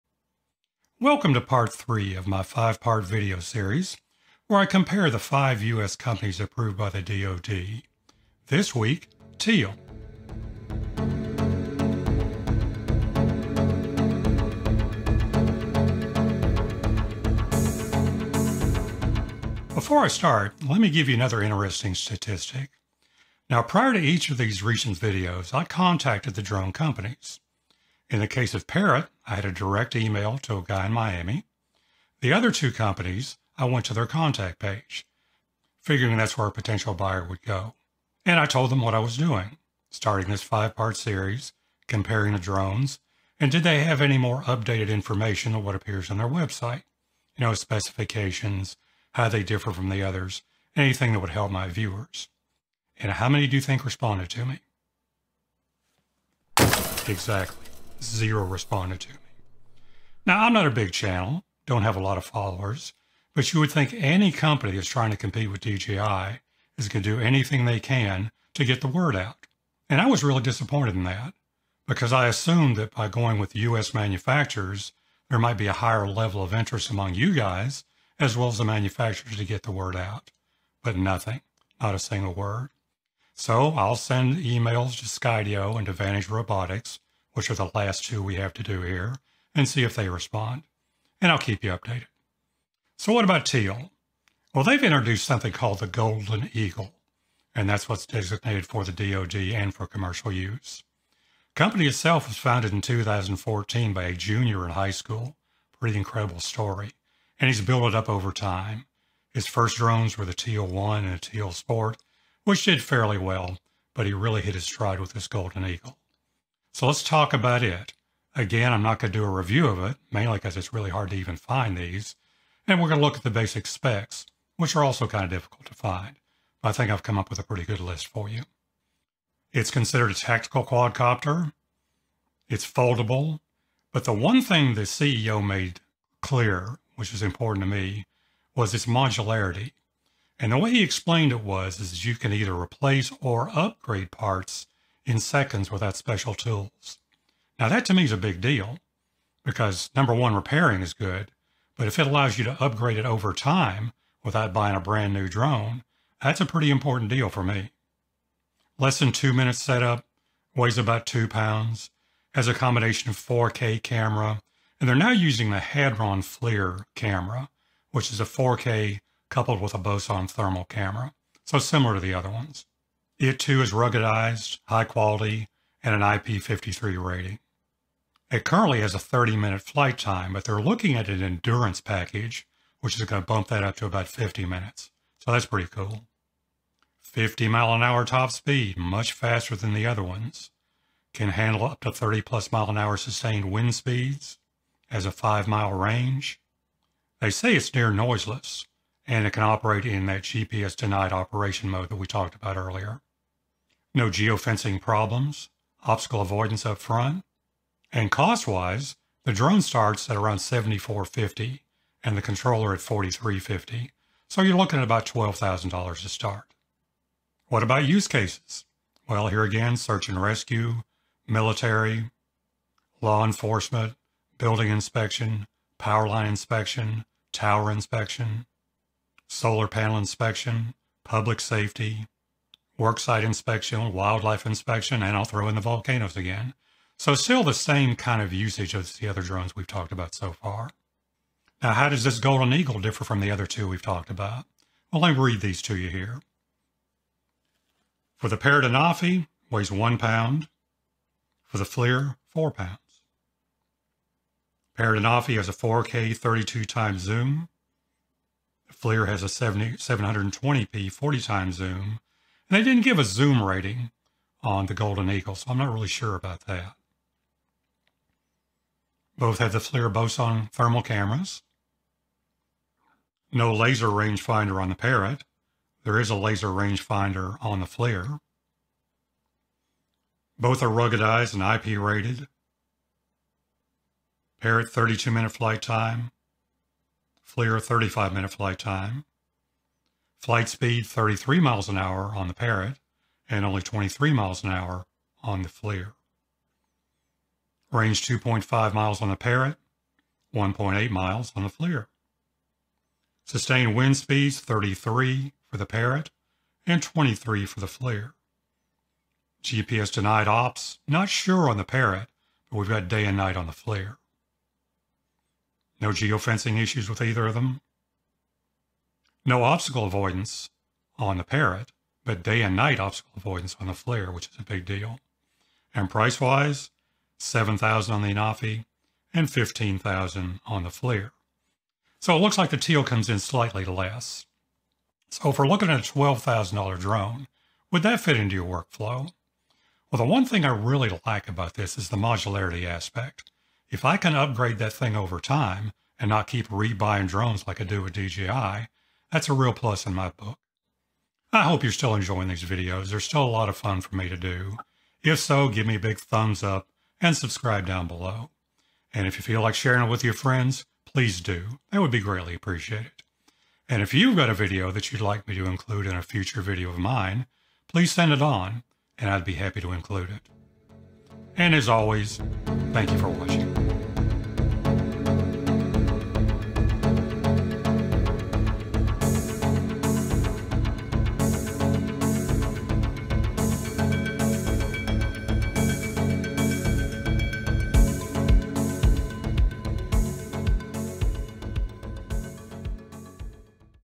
Teal Golden Eagle specs
This week I talk about the Teal Golden Eagle and how it compares to other Drones on the US DoD list of approved US Drone manufacturers.